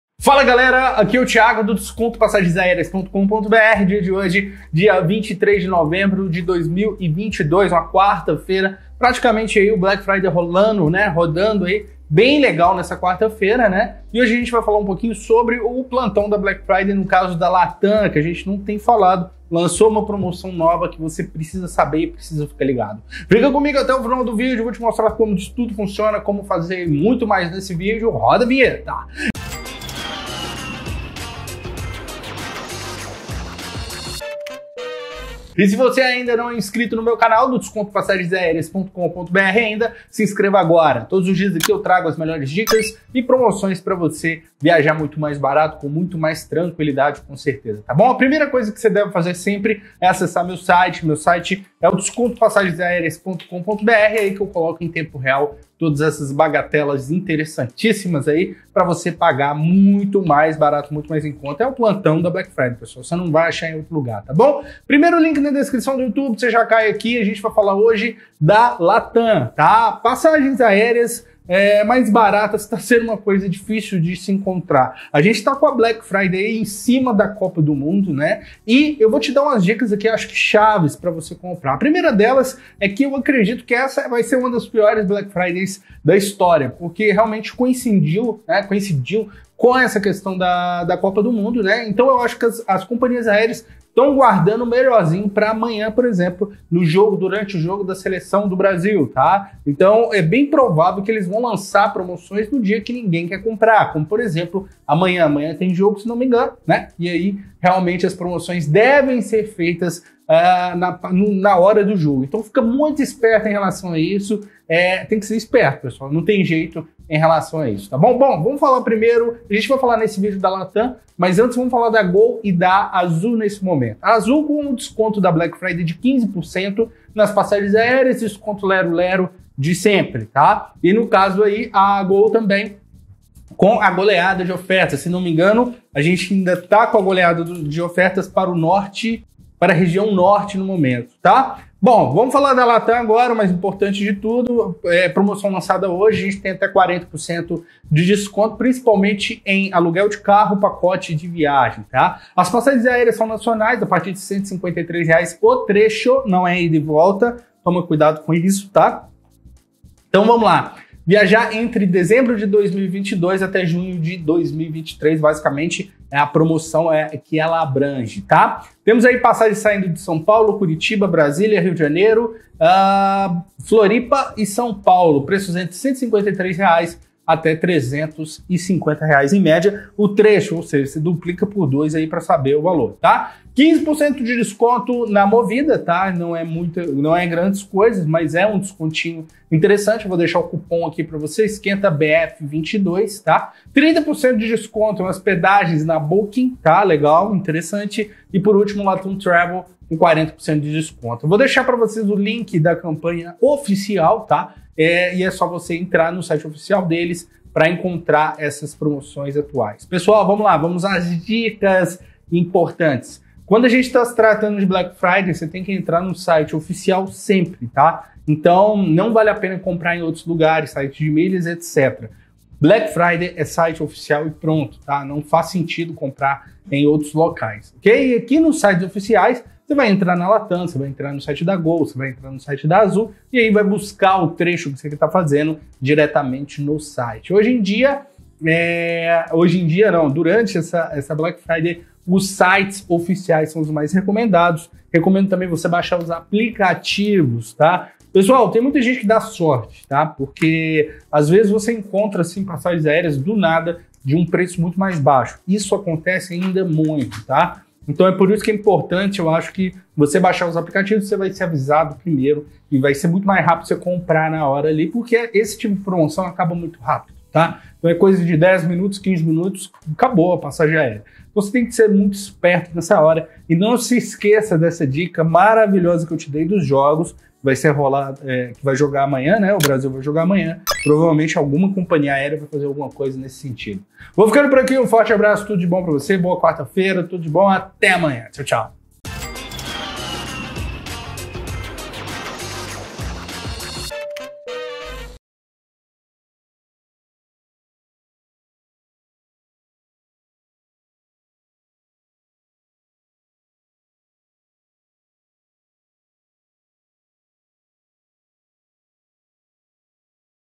BLACK FRIDAY LATAM ESPECIAL - 40% DE DESCONTO EM PACOTES, PASSAGENS AÉREAS E MUITO MAIS!
Olá viajantes do Brasil! As companhias aéreas já estão a todo o vapor com suas promoções especiais de Black Friday! A LATAM já inicío seu Esquenta Black Friday e tem diversas promoções em seu site oficial. Você vai encontrar passagens aéreas baratas, cupom de desconto em aluguel de carro, pacotes com 40% de desconto e muito mais. Confira:

Tabela de Conteúdo
Passagens nacionais a partir de R$153
Ganhe 15% de Desconto em aluguel de carro na Movida
30% de Desconto em Hospedagens na Booking/LATAM TRAVEL
Pacotes LATAM TRAVEL com até 40% de desconto
As passagens aéreas mais baratas de hoje é encontrada na LATAM. Os trechos estão a partir de R$153 com taxas de embarque inclusas. Os voos promocionais são de tarifa econômica e para viajar entre dezembro/22 a junho/23. Confira abaixo alguns trechos em promoção no Esquenta Black Friday LATAM:

Saindo de São Paulo – Para: São José do Rio Preto – Trechos a partir de R$153
Saindo de Curitiba – Para: Rio de Janeiro – Trechos a partir de R$167
Saindo de Brasília – Para: Campo Grande – Trechos a partir de R$177
Saindo de Rio de Janeiro – Para: Belo Horizonte – Trechos a partir de R$241
Saindo de Curitiba – Para: Foz do Iguaçu – Trechos a partir de R$254
Saindo de São Paulo – Para: Navegantes – Trechos a partir de R$259
Saindo de Rio de Janeiro – Para: Vitória – Trechos a partir de R$288
Saindo de Curitiba – Para: São Paulo – Trechos a partir de R$294
Saindo de Brasília – Para: Foz do Iguaçu – Trechos a partir de R$296
Saindo de Brasília – Para: Uberlândia – Trechos a partir de R$299
Saindo de Florianópolis – Para: São Paulo – Trechos a partir de R$305
Saindo de São Paulo – Para: Passo Fundo – Trechos a partir de R$320
Estas são algumas promoções que estão acontecendo hoje na LATAM e tem muitos outros trechos em promoção. Clique aqui e confira todas as promoções de passagens aéreas nacionais da Black Friday LATAM.

Ganhe 15% de desconto nas diárias de veículos de grupo F/J/K/KX/L/MJ aplicando o cupom ESQUENTABF22. A promoção é uma parceria entre a LATAM TRAVEL e Movida. A reserva deve ser feita pelo site da LATAM TRAVEL até dia 24 de novembro com retirada do veículo entre 28/nov/2022 a 14/dez/2022.

30% de Desconto em Hospedagens na Booking/LATAM TRAVEL
A LATAM TRAVEL está com parceria com a Booking e você pode ganhar 30% de desconto reservando seu hotel. Nessa promoção você também acumula pontos LATAM PASS para subir de categoria. Na reserva do seu hotel com LATAM Travel em parceria com Booking.com ganhe 30 Pontos Latam Pass e 6 pontos qualificáveis a cada US$ 1,00 gasto,

A promoção é válida para compras realizadas até dia 25 de novembro. O período da hospedagem é de 7/nov/2022 e 31/dez/2023 para destinos nacionais e Internacionais.

Os pacotes da LATAM TRAVEL estão com até 40% de desconto. O desconto é válido para pacotes que incluem passagem aérea de ida e volta e hospedagem em apartamento duplo de categoria standard. São diversos pacotes nacionais e internacionais disponíveis para viagens entre dezembro/22 a junho/23.

A promoção não abrange viagens a serem realizadas em períodos de feriados, congressos, feiras ou eventos. Essa promoção é só até hoje, dia 23 de novembro. Compre já!

Confira todas essas promoções da Black Friday LATAM aqui!